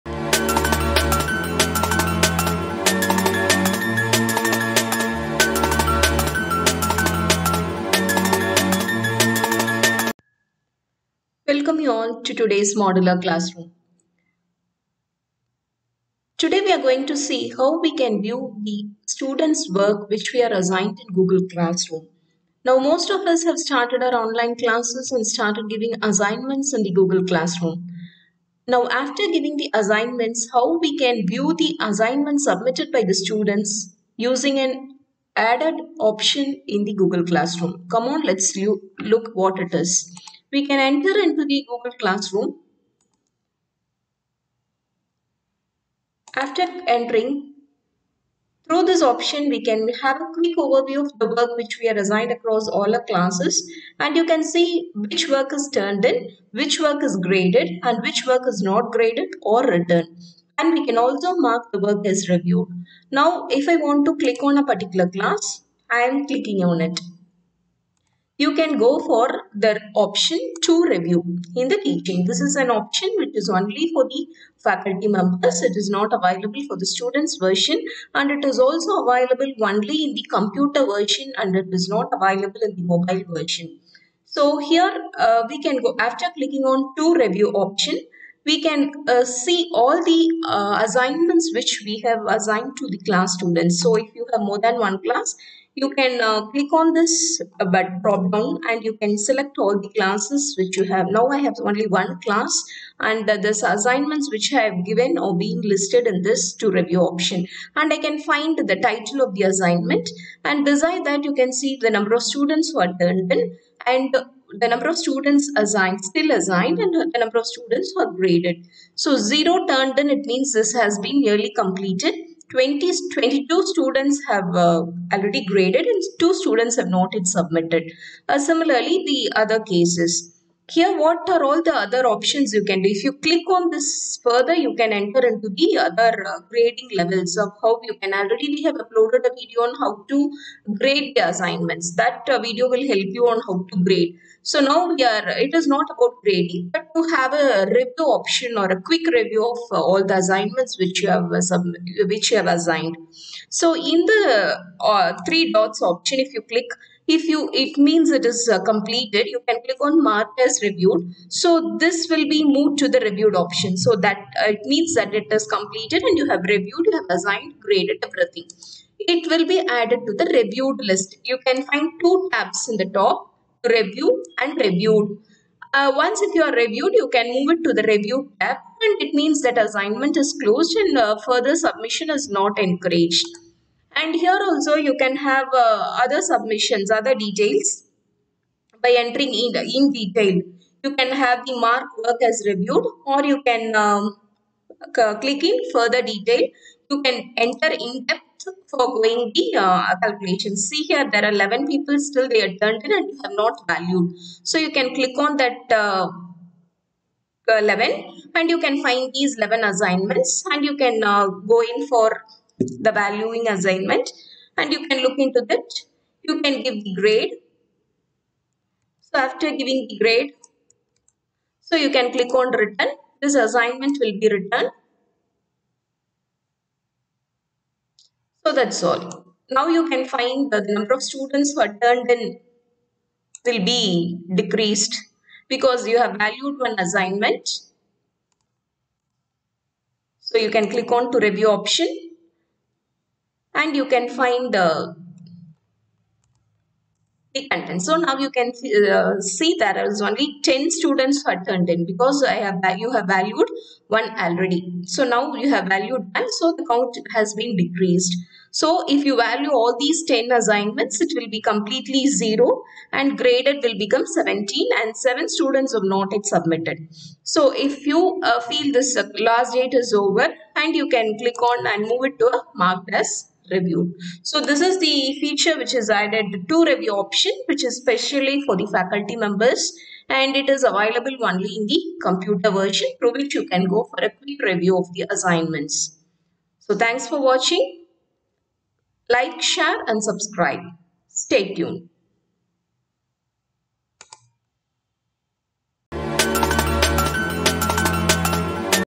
How to Review the Assignment in Google Classroom?
Quick Review of Assignments
Check  for Turned-In, Assigned and Graded